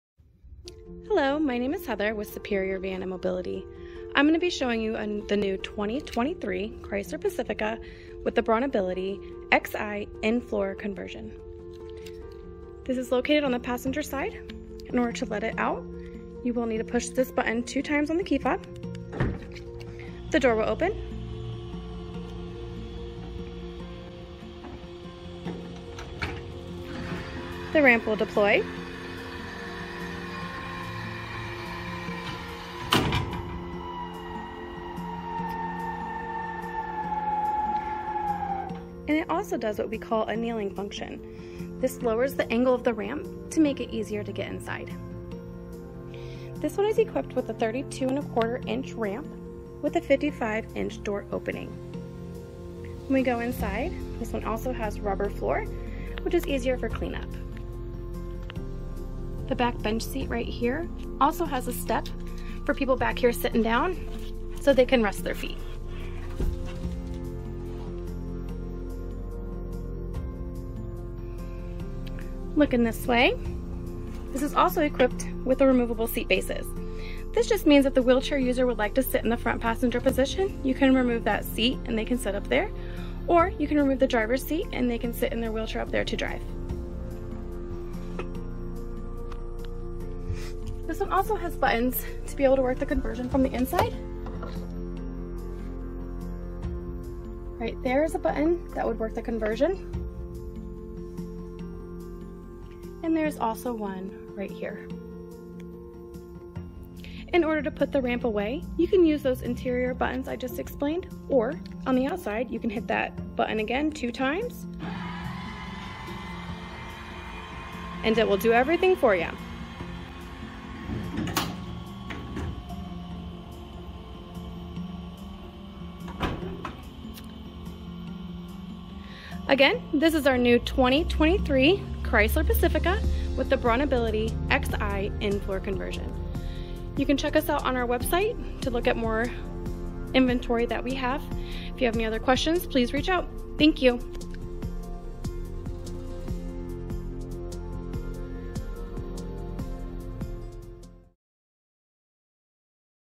Demo - 2023 Chrysler Pacifica Wheelchair Van With In-Floor Ramp from BraunAbility
This short video shows the operation of an in-floor wheelchair van conversion on a 2023 Chrysler Pacifica from BraunAbility.

The conversion features a 32.25" wide ramp with a 1000 lbs. load capacity and a 55" door opening height.

To learn more about this or other types of wheelchair-accessible vehicles, be sure to check out our